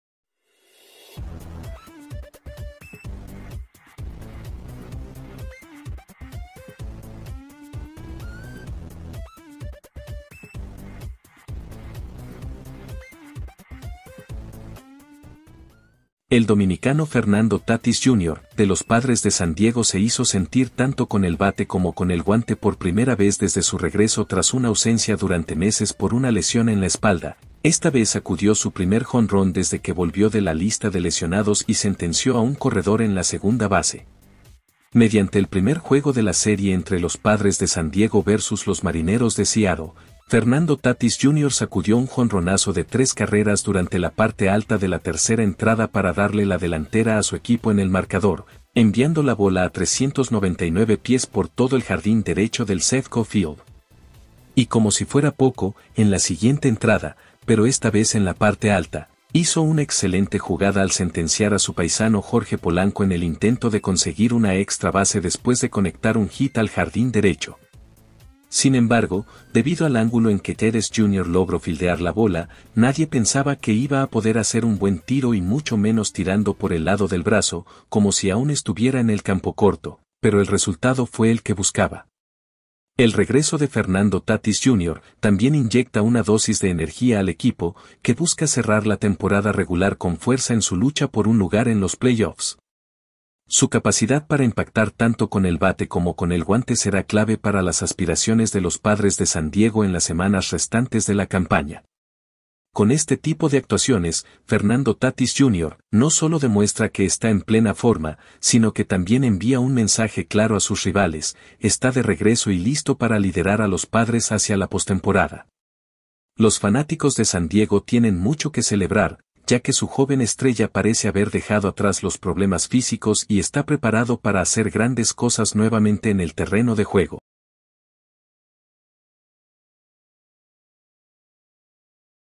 Fernando Tatis Jr. Sacude JONRONAZO y Enseña a Respetar a un Paisano con tiro inesperado
El dominicano Fernando Tatis Jr. de los Padres de San Diego se hizo sentir tanto con el bate como con el guante por primera vez desde su regreso tras una ausencia durante meses por una lesión en la espalda, esta vez sacudió su primer jonrón desde que volvió de la lista de lesionados y sentenció a un corredor en la segunda base.